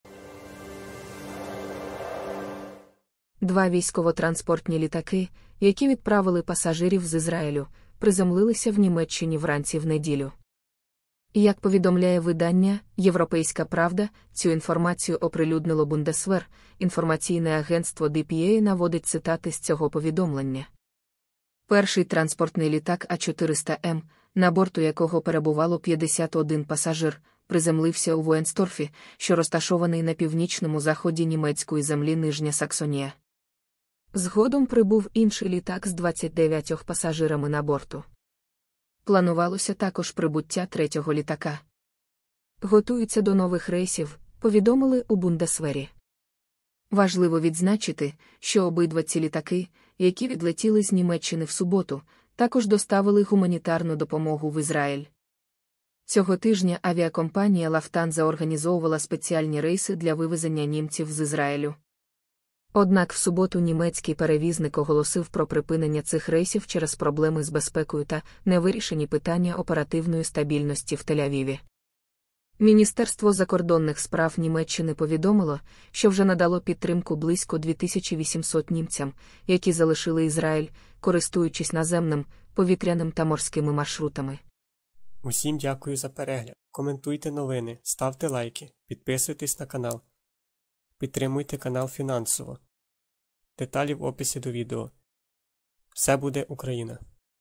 Транспортні літаки A400M ВПС Німеччини залучили для евакуації громадян Німеччини з Ізраїлю
Фото: DPA

Джерело originally posted to Flickr as A400M
Автор Chris

Источник commons file
Автор
Peng Chen

Источник Airbus A-400M Atlas
Автор Ronnie Macdonald from Chelmsford and Largs, United Kingdom

Источник Собственная работа
Автор Calibrux